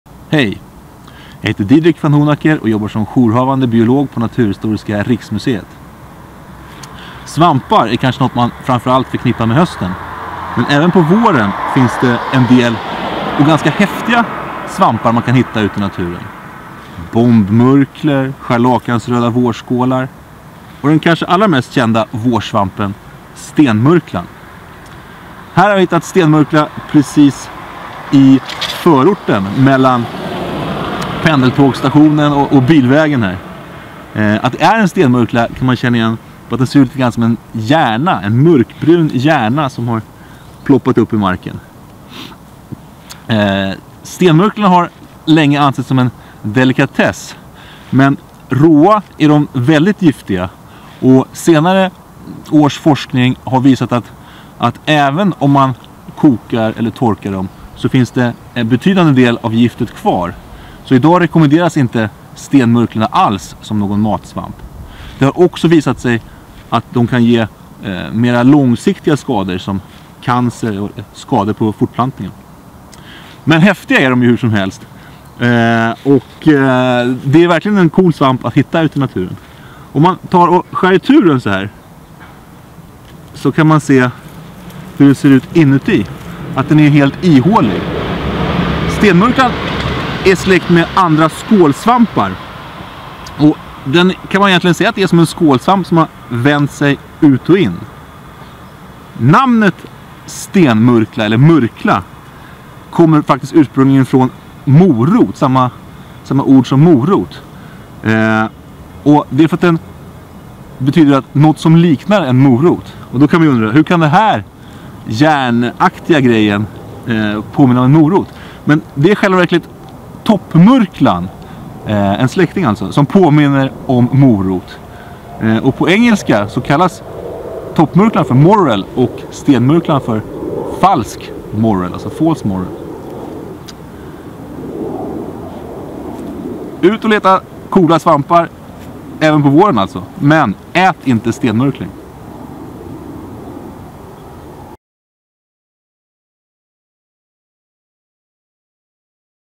Stenmurklor är coola, men giftiga - Aktuellt i naturen 16 april 2015
Didrik Vanhoenacker, museets jourhavande biolog, har hittat en stenmurkla vid pendeltågsstationen. Han berättar att de idag anses så giftiga att man inte alls bör äta dem. Visste du förresten var ordet murkla kommer ifrån?